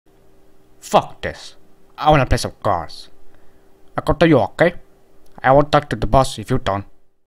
Impersonating Tony Montana Ep. 1
I was training my voice to be like this, somewhy in the person it sound better. But maybe in 3 years... I'm gonna be a fuckin' mastermind.